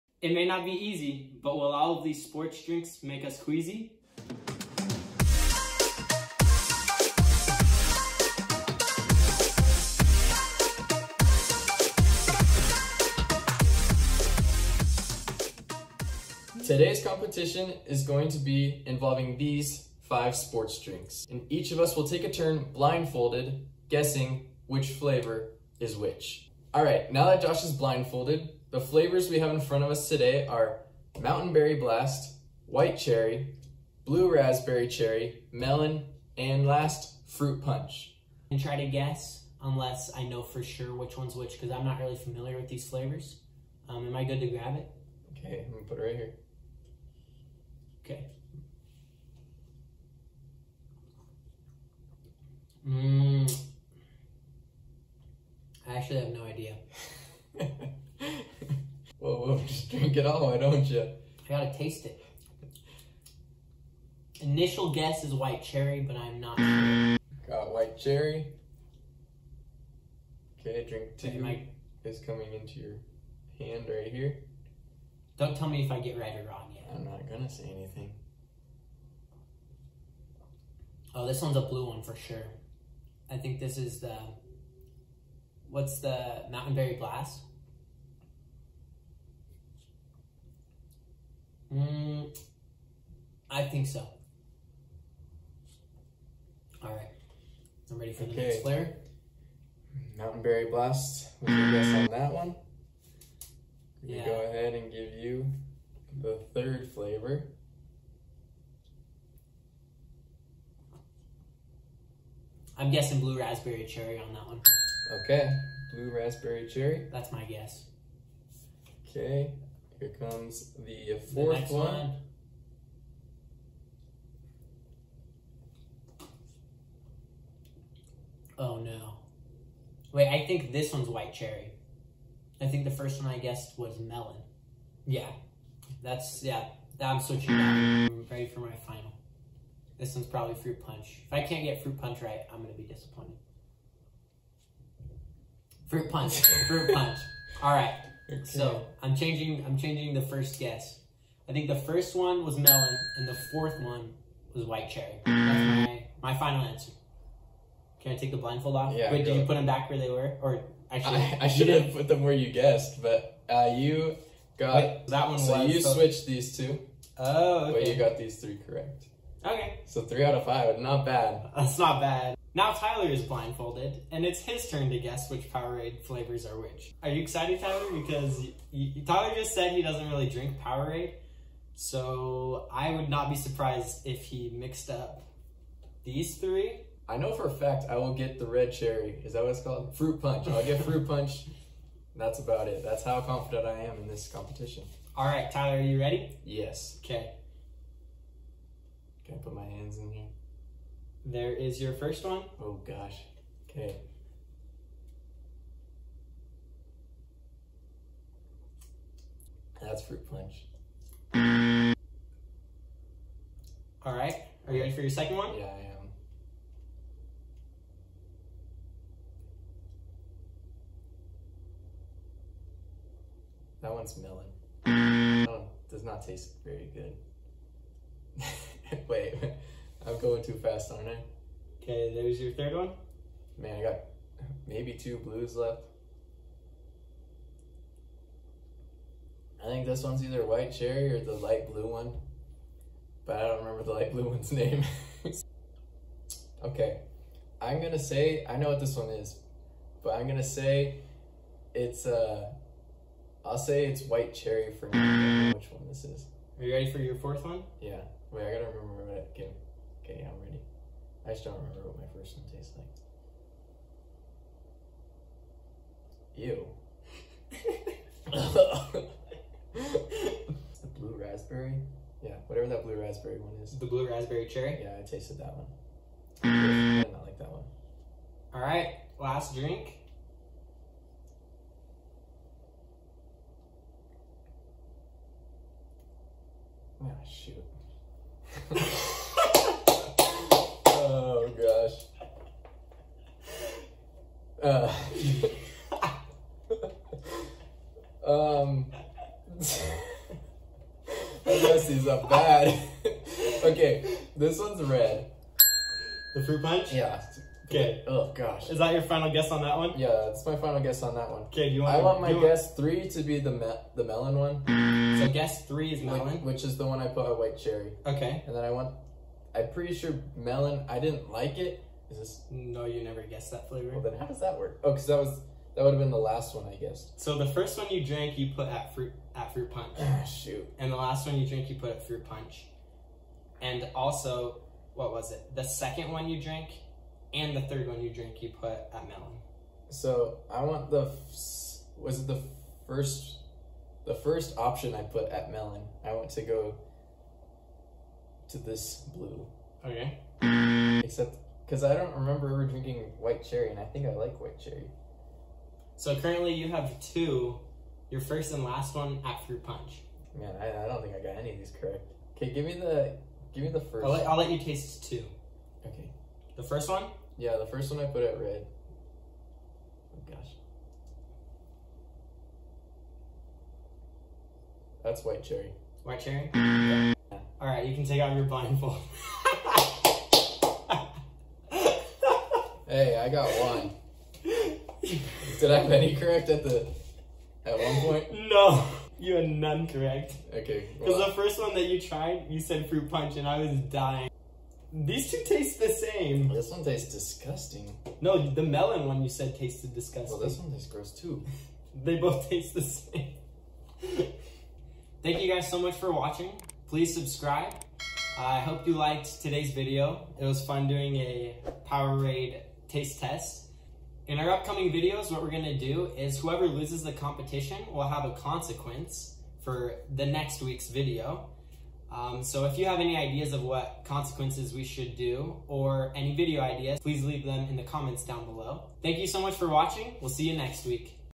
ULTIMATE Sports Drink Taste Test
Can we guess which one is which? We took a bit of a break from the normal sports videos to try something new out. Let us know if you guys like these kinds of video because they are fun to film.

Just remember that Powerade is not a sponsor unless they magically see this video a want to be.